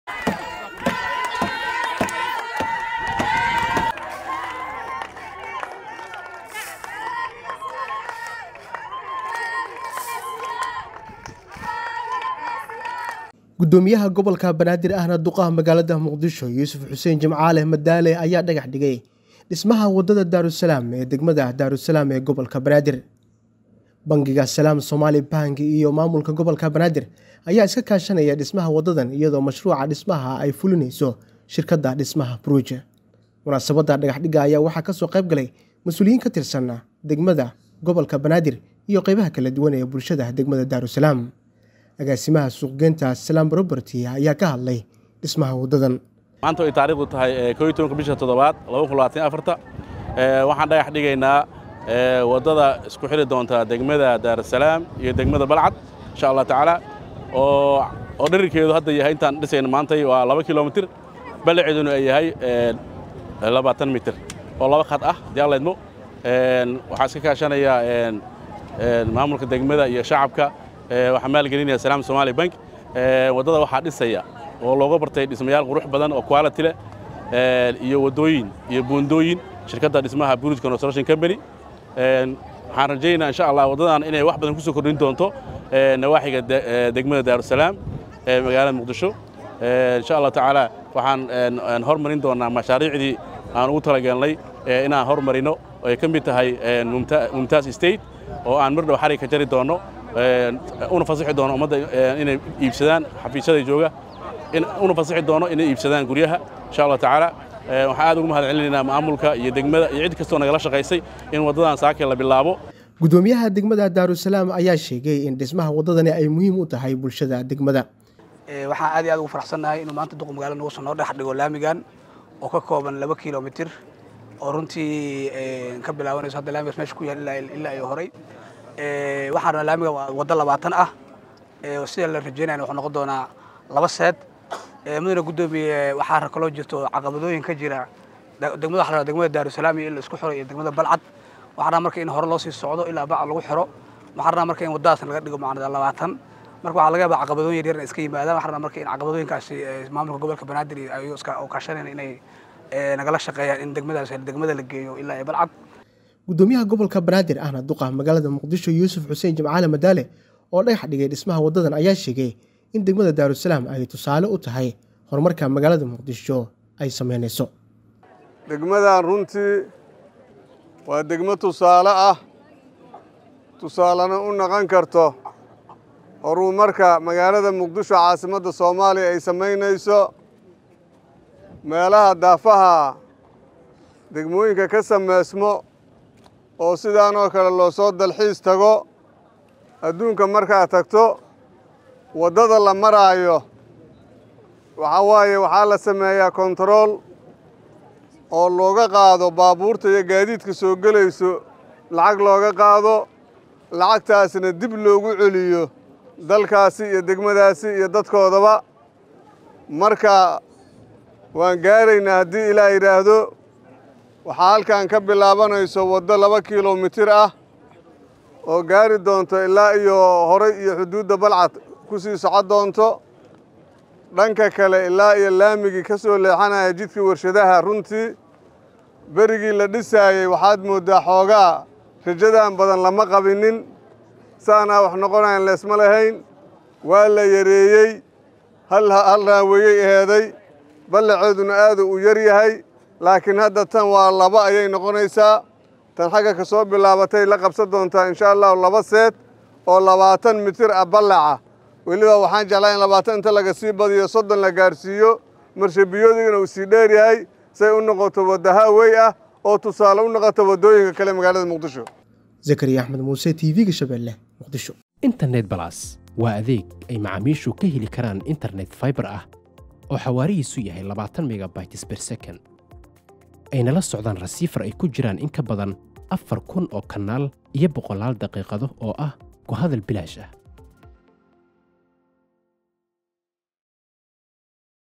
Gudoomiye Madaale oo dhagax dhigay dhismaha wadada Daaru-salaam oo isku xiri doonto.................
Gudoomiye Madaale oo dhagax dhigay dhismaha wadada Daaru-salaam oo isku xiri doonto Muqdisho iyo Balcad.